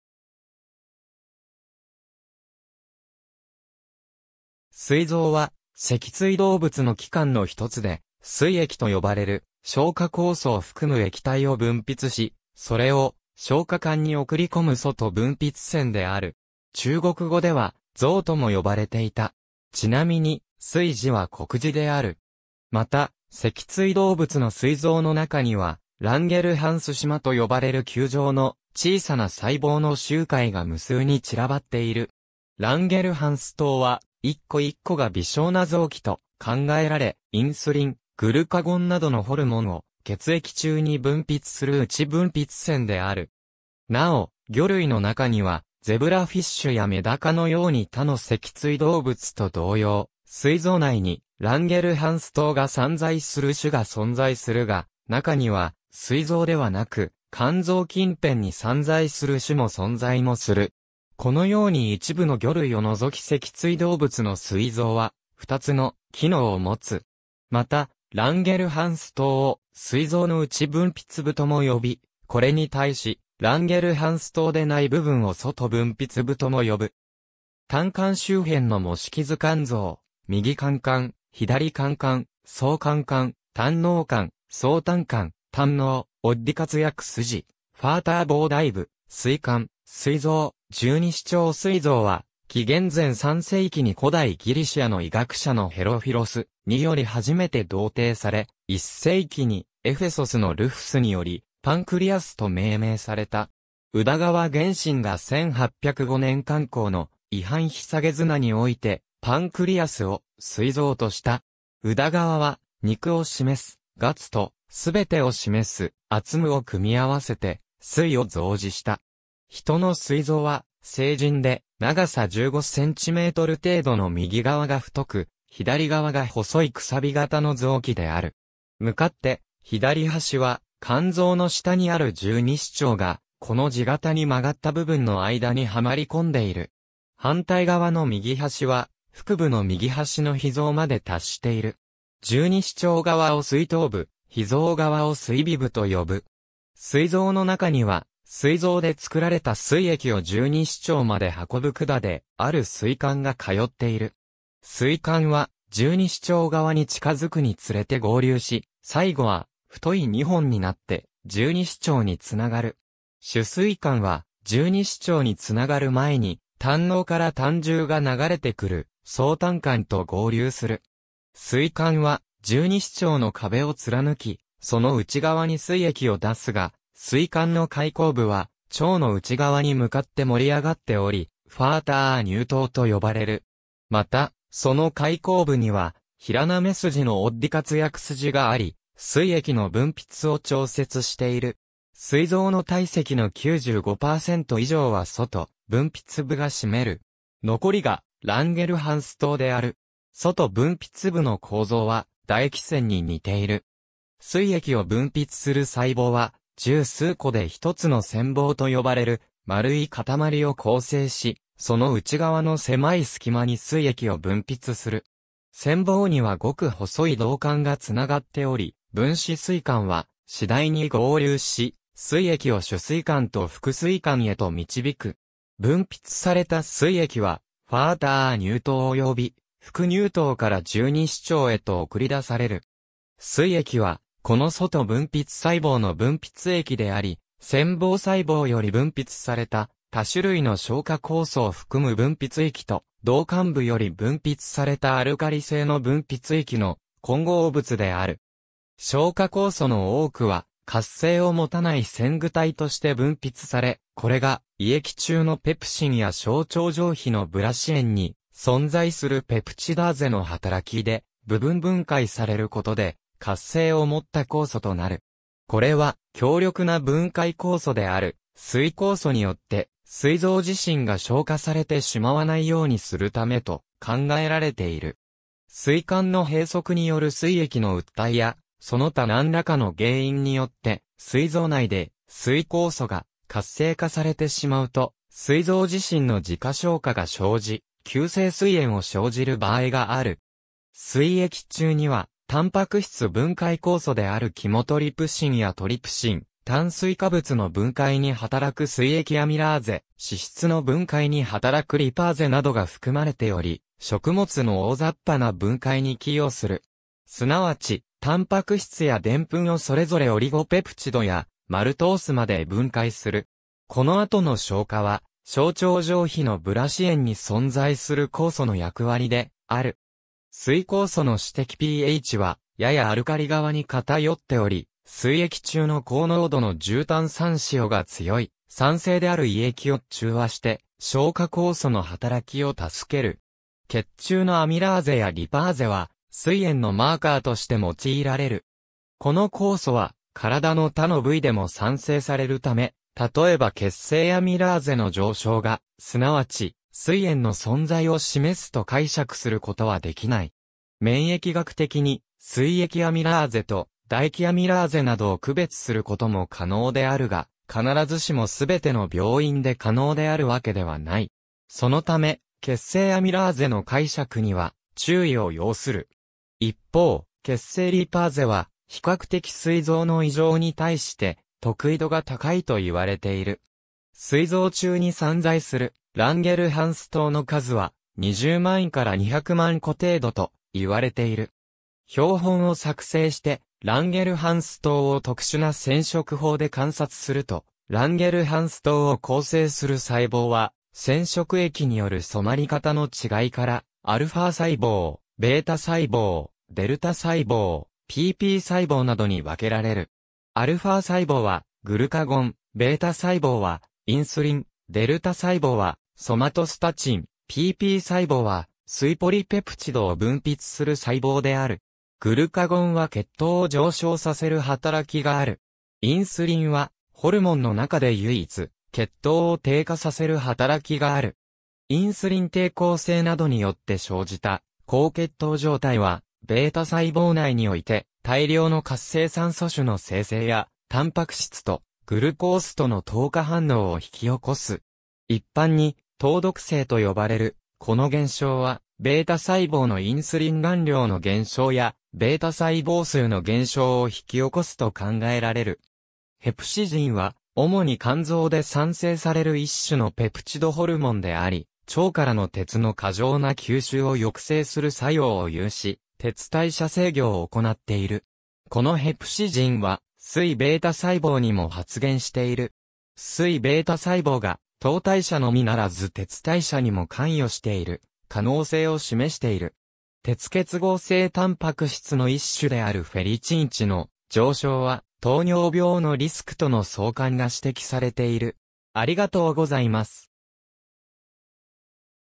膵臓
膵臓（すいぞう、英: pancreas）は、脊椎動物の器官のひとつで、膵液と呼ばれる消化酵素を含む液体を分泌し、それを消化管に送り込む外分泌腺である。
中国語では「胰臓（いぞう）」とも呼ばれていた。
因みに「膵」字は国字である。
また、脊椎動物の膵臓の中には、ランゲルハンス島（らんげるはんすとう）と呼ばれる球状の小さな細胞の集塊が無数に散らばっている。
ランゲルハンス島は、1個1個が微小な臓器と考えられ、インスリン、グルカゴンなどのホルモンを血液中に分泌する内分泌腺である。
なお、魚類の中にはゼブラフィッシュやメダカのように他の脊椎動物と同様、膵臓内にランゲルハンス島が散在する種が存在するが、中には膵臓ではなく肝臓近辺に散在する種も存在もする。
このように一部の魚類を除き脊椎動物の膵臓は、2つの機能を持つ。
また、ランゲルハンス島を膵臓の内分泌部とも呼び、これに対し、ランゲルハンス島でない部分（膵液を分泌する部分）を外分泌部とも呼ぶ。
胆管周辺の模式図 肝臓、右肝管、左肝管、総肝管、胆嚢管、総胆管、胆嚢、オッディ括約筋、ファーター膨大部、膵管、膵臓、十二指腸 膵臓は紀元前3世紀に古代ギリシアの医学者のヘロフィロスにより初めて同定され、1世紀にエフェソスのルフスによりpancreasと命名された。
(pan-は「全て」、-creasは「肉」の意) 宇田川玄真が1805年刊行の『医範提綱』において"pancreas"を「膵臓」とした。
宇田川は「肉」を示す「月」と、「全て」を示す「萃」を組み合わせて『膵』を造字した。
ヒトの膵臓は、成人で長さ15 cm程度の右側が太く左側が細いくさび型の臓器である。
向かって左端は、肝臓の下にある十二指腸がコの字型に曲がった部分の間にはまりこんでいる。
反対側の右端は、腹部の右端の脾臓まで達している。
十二指腸側を膵頭部、脾臓側を膵尾部と呼ぶ。
膵臓の中には、膵臓で作られた膵液を十二指腸まで運ぶ管である膵管が通っている。
膵管は、十二指腸側に近づくにつれて合流し、最後は太い2本（主膵管、副膵管）になって、十二指腸につながる。
主膵管は、十二指腸につながる前に胆嚢から胆汁が流れてくる総胆管と合流する。
膵管は十二指腸の壁を貫き、その内側に膵液を出すが、膵管の開口部は腸の内側に向かって盛り上がっており、ファーター乳頭（十二指腸乳頭）と呼ばれる。
また、その開口部には平滑筋のオッディ括約筋があり膵液の分泌を調節している。
膵臓の体積の95%以上は外分泌部が占める。
残りがランゲルハンス島である。
外分泌部の構造は、唾液腺に似ている。
膵液を分泌する細胞は、十数個でひとつの腺房と呼ばれる丸い塊を構成し、その内側のせまい隙間に膵液を分泌する。
腺房にはごく細い導管（分枝膵管）がつながっており、分枝膵管は次第に合流し、膵液を主膵管と副膵管へと導く。
分泌された膵液はファーター乳頭（大十二指腸乳頭）および副乳頭（小十二指腸乳頭）から十二指腸へと送り出される。
膵液は、この外分泌細胞の分泌液であり、腺房細胞より分泌された多種類の消化酵素を含む分泌液と、導管部より分泌されたアルカリ性の分泌液の混合物である。
消化酵素の多くは活性を持たない前駆体（例えばキモトリプシノーゲン）として分泌され、これが胃液中のペプシンや小腸上皮の刷子縁に存在するペプチダーゼの働きで部分分解される事で、活性を持った酵素（例えばキモトリプシン）となる。
これは、強力な分解酵素である膵酵素によって膵臓自身が消化されてしまわないようにするためと考えられている。
膵管の閉塞による膵液の鬱滞やその他何らかの原因によって膵臓内で膵酵素が活性化されてしまうと、膵臓自身の自家消化が生じ、急性膵炎を生じる場合がある。
膵液中にはタンパク質分解酵素であるキモトリプシンやトリプシン、炭水化物の分解に働く膵液アミラーゼ、脂質の分解に働くリパーゼなどが含まれており、食物の大雑把な分解に寄与する。
すなわち、タンパク質やデンプンをそれぞれオリゴペプチド（ペプチド）やマルトースまで分解する。
この後の消化は小腸上皮の刷子縁に存在する酵素の役割である。
膵酵素の至適pHは、ややアルカリ側に偏っており、膵液中の高濃度の重炭酸塩が強い酸性である胃液を中和して消化酵素の働きを助ける。
血中のアミラーゼやリパーゼは膵炎のマーカーとして用いられる。
この酵素は、身体の他の部位でも産生されるため、例えば血清アミラーゼの上昇が、すなわち膵炎の存在を示すと解釈することはできない。
免...